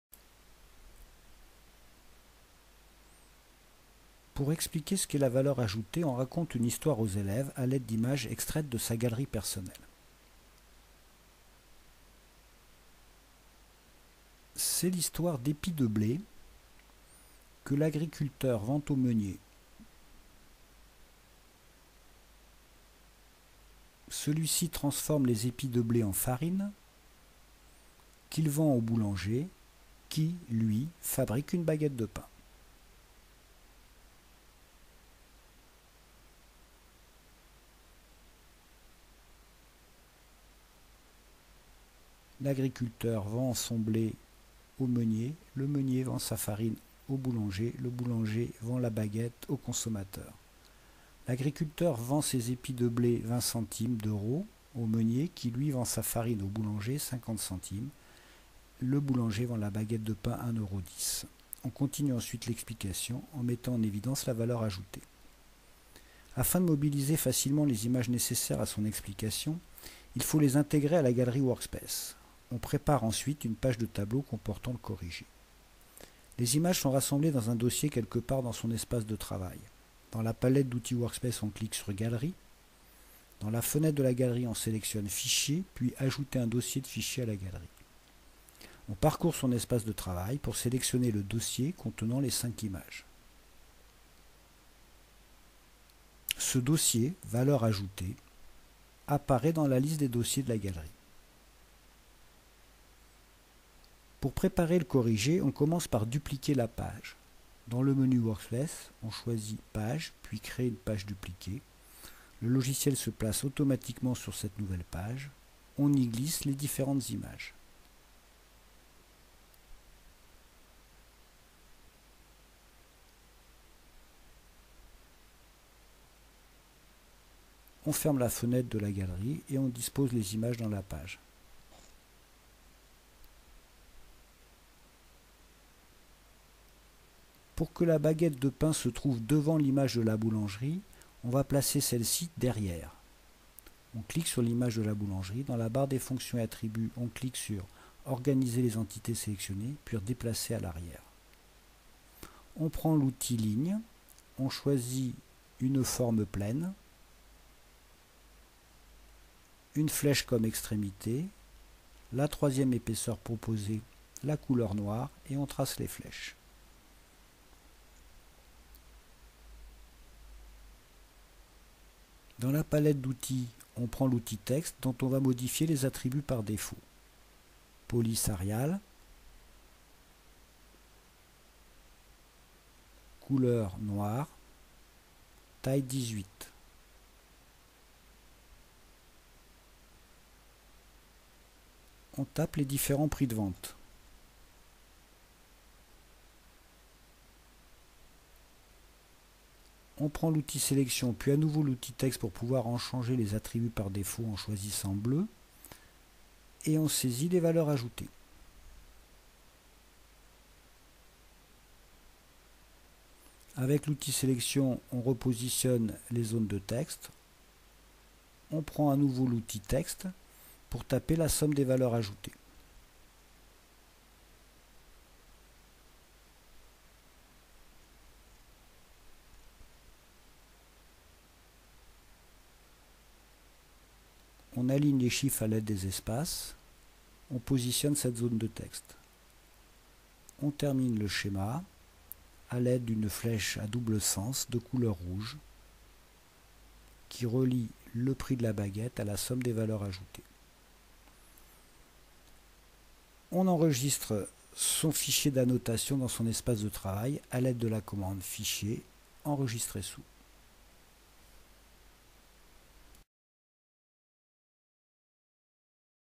Fiche 7 - La valeur ajoutée - Interwrite Workspace
Création en direct d'un schéma conceptuel
Improvisation préparée. En réunissant au préalable, dans sa {Galerie} personnelle, les ressources qu'il prévoit d'utiliser en classe, le professeur peut, au moment opportun, s'appuyer sur ces ressources pour construire sa démonstration. Ici, le concept de valeur ajoutée est présenté à partir de l'histoire d'un épi de blé qui devient farine puis baguette de pain.